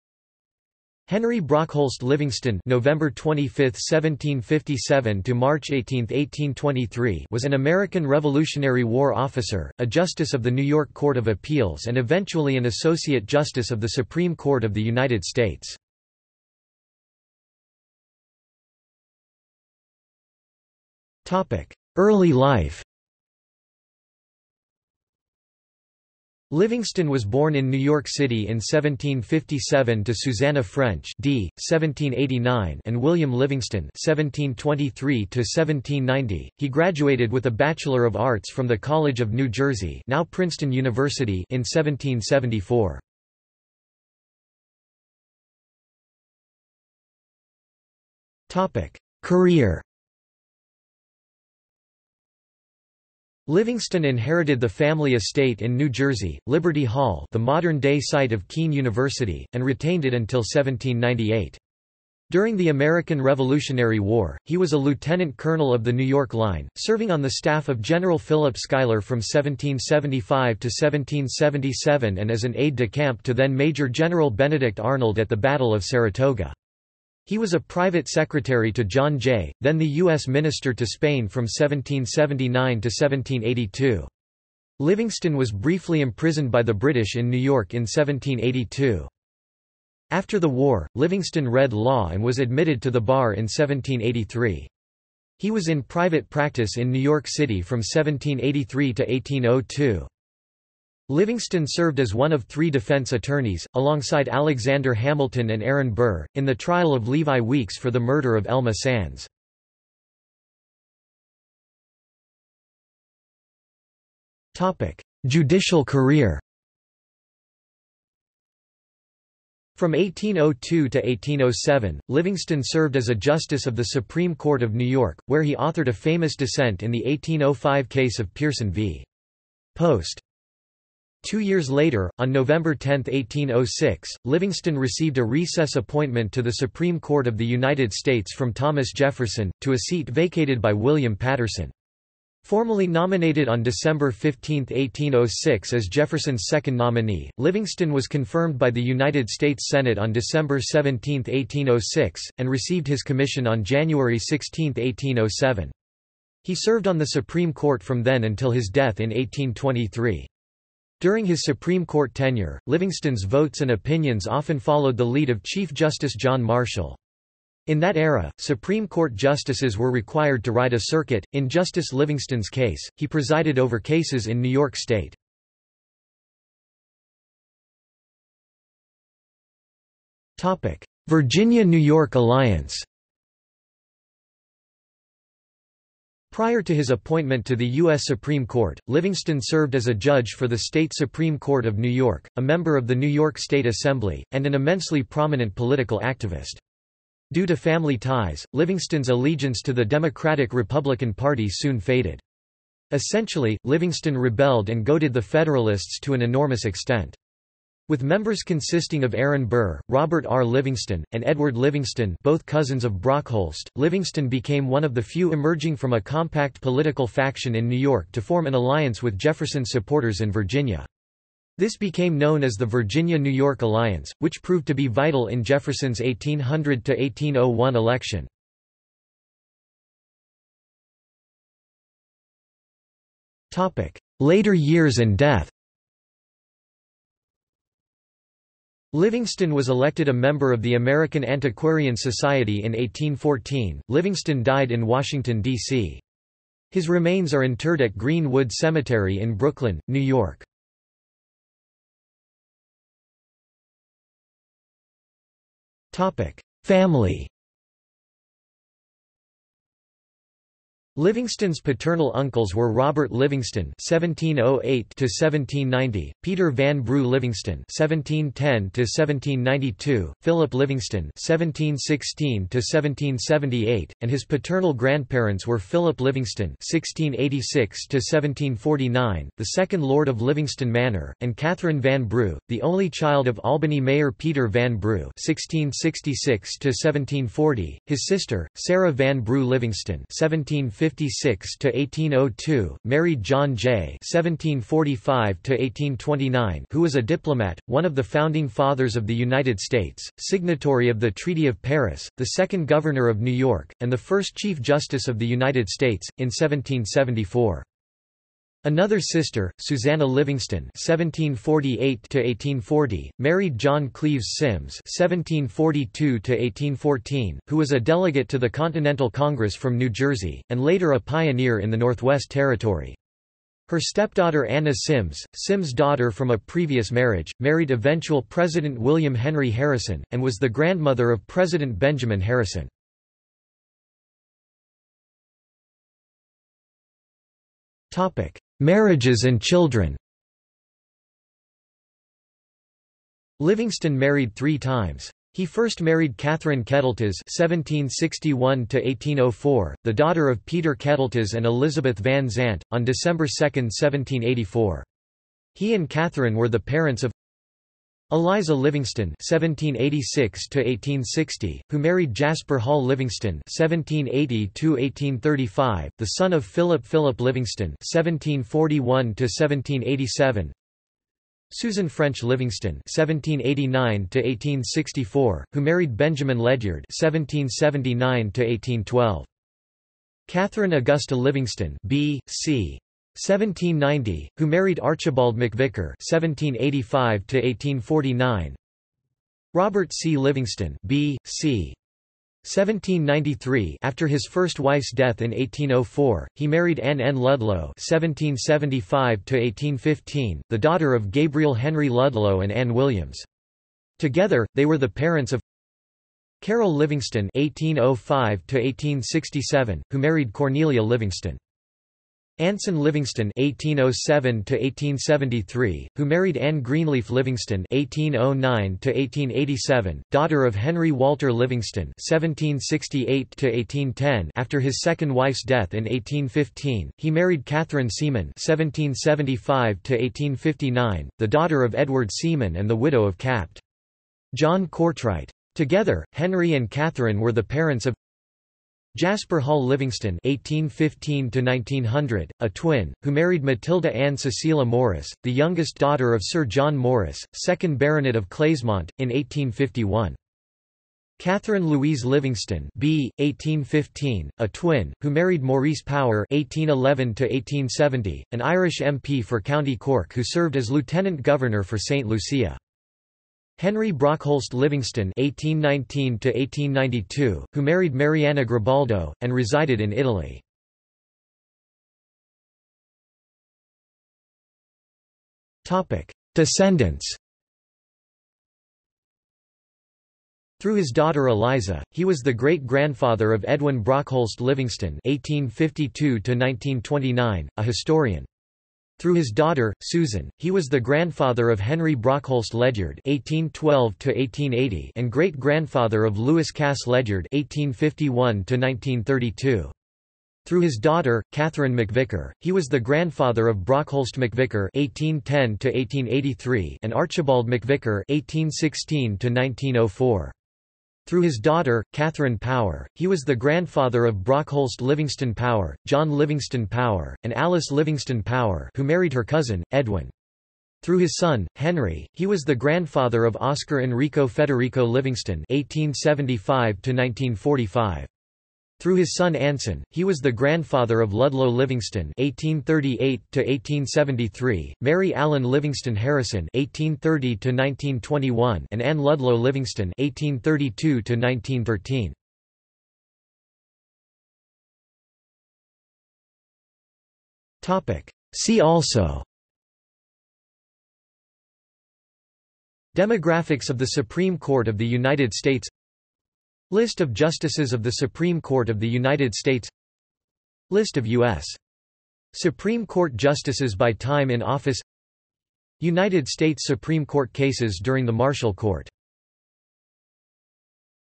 Henry Brockholst Livingston | Wikipedia audio article
This is an audio version of the Wikipedia Article:
Henry Brockholst Livingston

00:00:17 1 Early life
00:00:44 2 Career
00:01:51 2.1 Judicial career
00:02:57 2.2 Virginia-New York Alliance
00:03:50 2.3 Later years and death
00:04:12 3 Family
00:05:34 3.1 Marriages and children
00:07:52 3.2 Descendants
00:09:04 4 See also

- Socrates

SUMMARY
Henry Brockholst Livingston (November 25, 1757 – March 18, 1823) was an American Revolutionary War officer, a justice of the New York Court of Appeals and eventually an Associate Justice of the Supreme Court of the United States.